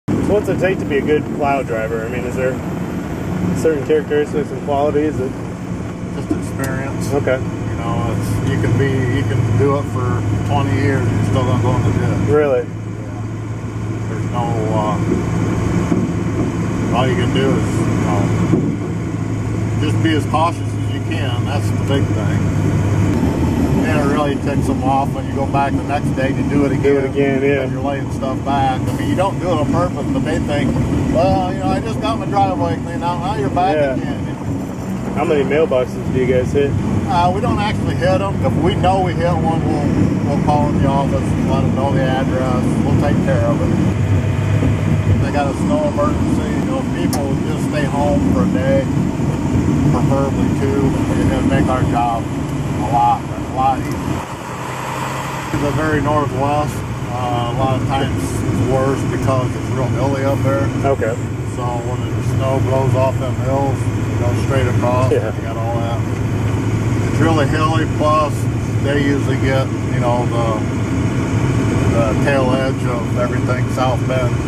Neighbors: Jim Pfefferkorn
Jim Pfefferkorn is a plow driver with the DeKalb County Highway Department, which means through all the torrential snowstorms and subzero temperatures and travel emergencies this winter has bestowed upon the region, he can be found perched in a yellow county plow truck forging paths through the elements.

Read more in Monday's KPC newspapers.